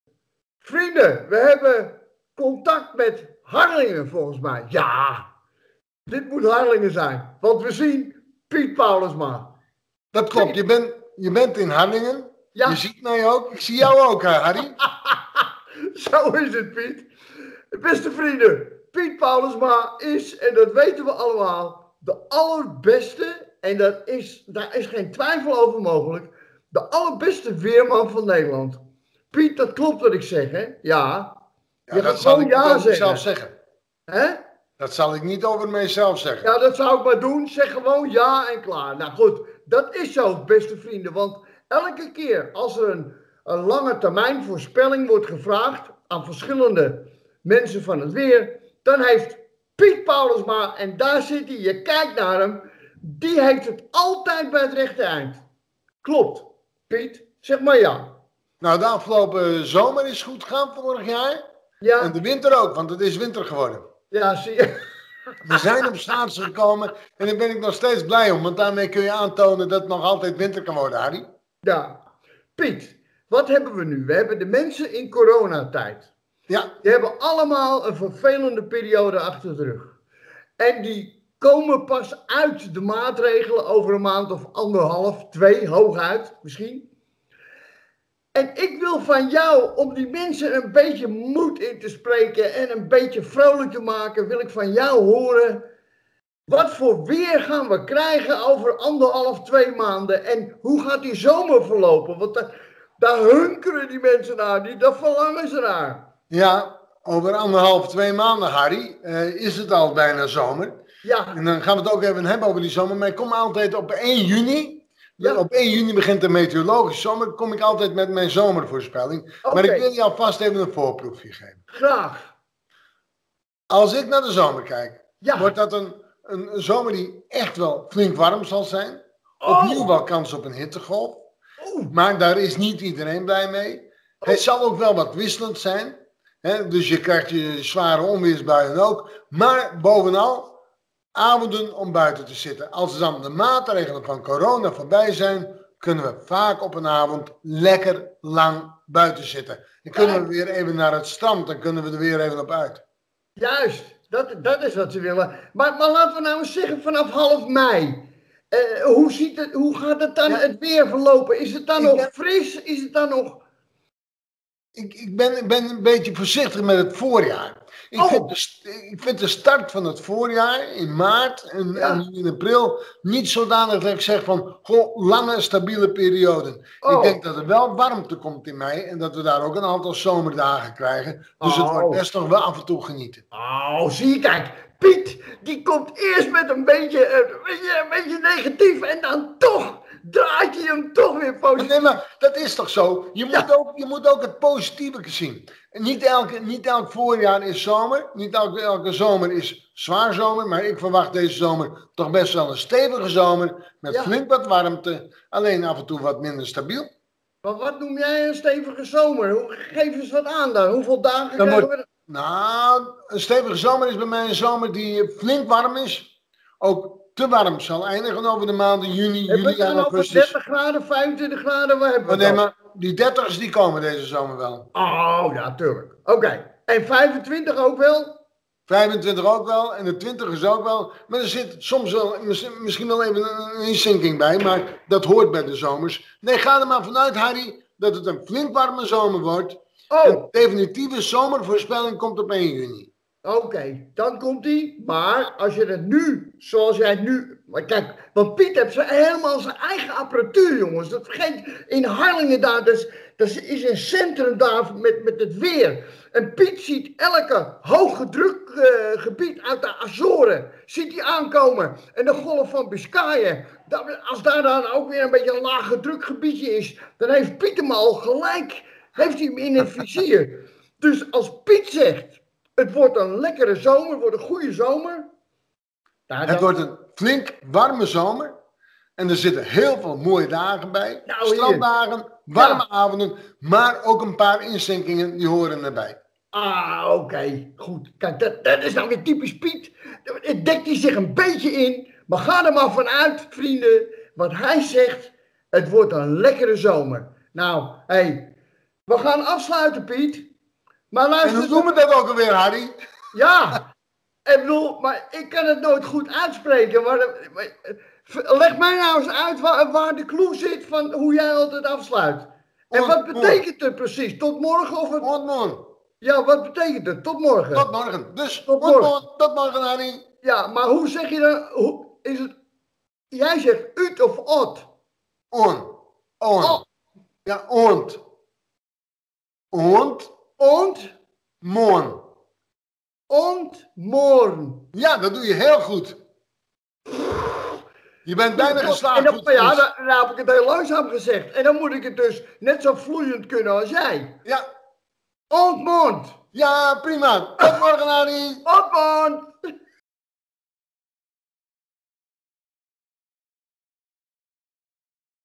Piet Paulusma voorspelt de zomer!
Piet Paulusma. FF skypen met de beste weerman van Nederland. Hij voorspelt de zomer. Na alle maatregelen zijn de Nederlanders toe aan een leven zonder beperkingen en lekker weer!